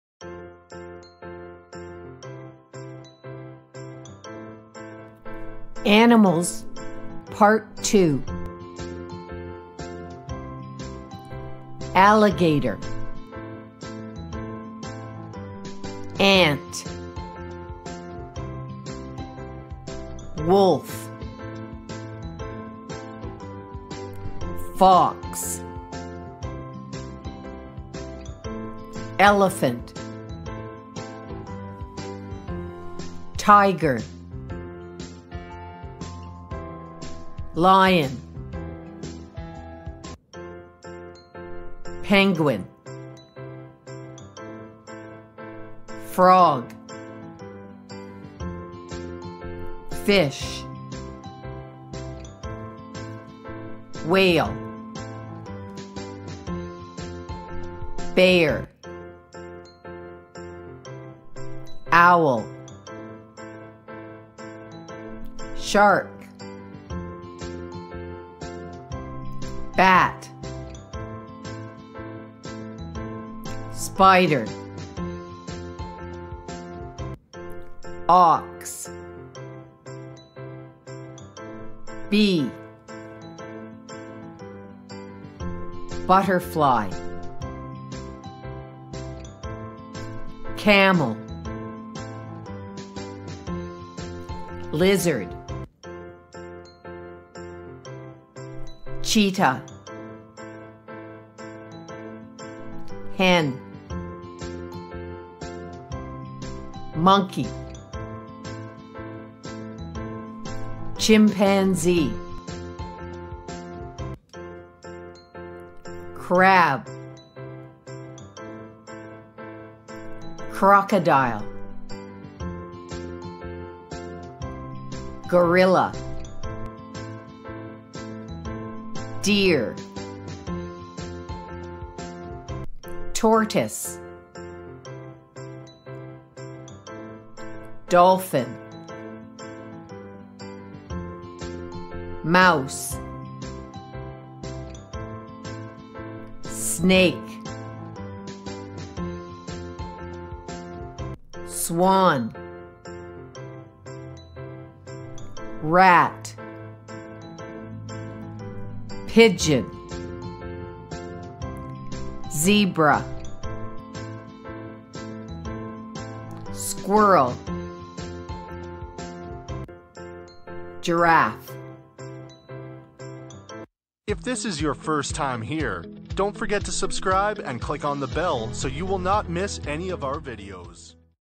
Animals, part 2
Enroll in our school for free and enjoy our grammar courses

00:00 to 00:28

Animals

Alligator / Ant / Wolf / Fox

00:29 to 00:45

Elephant / Tiger / Lion / Penguin

00:46 to 01:01

Frog / Fish / Whale / Bear

01:02 to 01:17

Owl / Shark / Bat / Spider

01:18 to 01:34

Ox / Bee / Butterfly / Camel

01:35 to 01:50

Lizard / Cheetah / Hen / Monkey

01:51 to 02:08

Chimpanzee / Crab / Crocodile / Gorilla

02:09 to 02:25

Deer / Tortoise / Dolphin / Mouse

02:26 to 02:41

Snake / Swan / Rat / Pigeon

02:43 to 03:02

Zebra / Squirrel / Giraffe